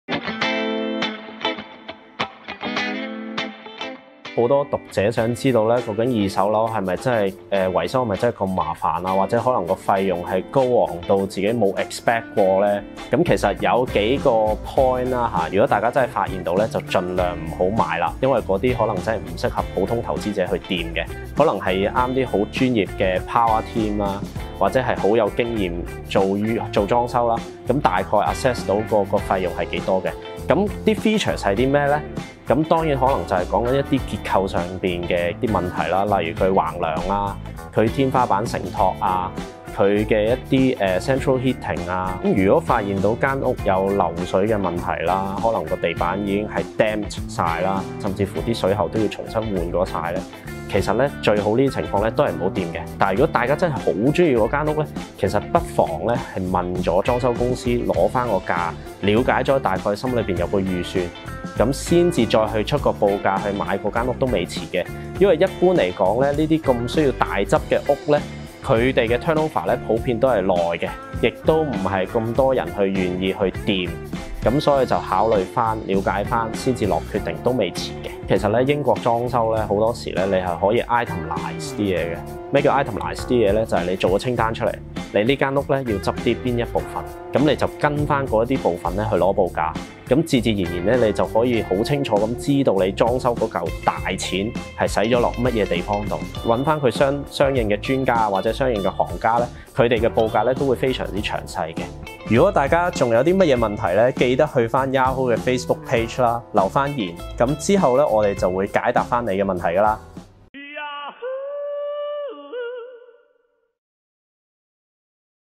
【英國買樓】買二手樓擔心要畀天價維修費？有咩樓盤應該敬而遠之？ | Yahoo Hong Kong
上集提到在英國購買二手樓要留意維修費用的問題，今集Smart2Go英國置業助理Derek Yip 便會簡單解說有甚麼樓盤應該敬而遠之。

Derek指若發現心水二手樓盤的樓宇結構上有問題，例如：橫樑、天花板承托力、中央暖氣、漏水、須更換全屋水管等等的大問題，便要小心了，因為這些樓宇未必適合普通投資者，可能是適合專業團隊或是熟悉裝修的行內人，因為他們能大概估算得到維修費用。

Derek續指如果你真的非常喜歡某個二手樓盤，不妨先向裝修公司報價，心中先有維修費用的預算才再出價也不遲，因為這些需要大裝修的樓盤交易時間一般都會較耐，因為不會太多人有興趣，可以先了解再下決定都未遲。另外，英國裝修是可以寫一張清單列明每個項目，你的樓盤要裝修什麼部分便根據不同的項目報價，一目了然。

👉🏻 觀看更多：
特別平嘅二手樓，維修費用可能要成百萬？！買二手樓有咩要注意？